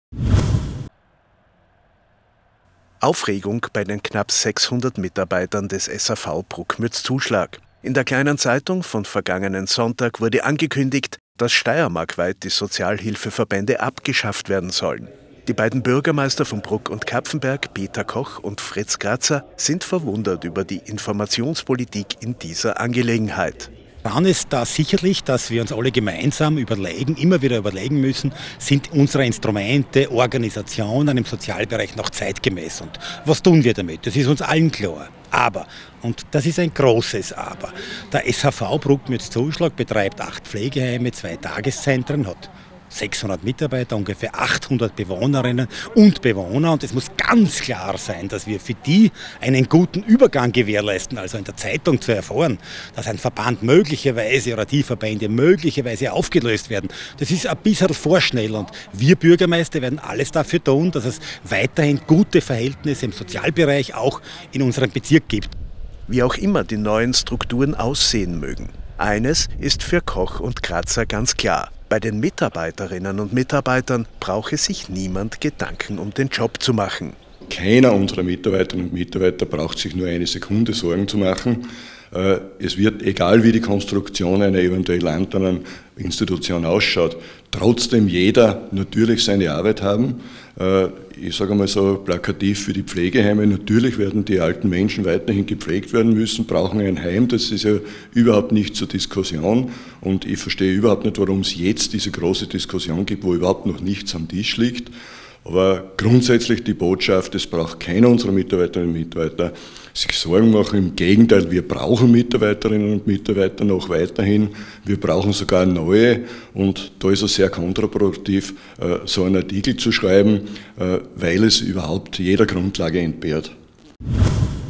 Aufregung beim SHV Bruck-Mürzzuschlag
In einem Zeitungsartikel vom vergangenen Sonntag wurde angekündigt das die Sozialhilfeverbände steiermarkweit abgeschafft werden sollen. Die Bürgermeister Peter Koch (Bruck) und Fritz Kratzer (Kapfenberg) zeigen sich verwundert über die Informationspolitik in dieser Sache und betonen: "Die Mitarbeiter brauchen sich keine Sorgen zu machen."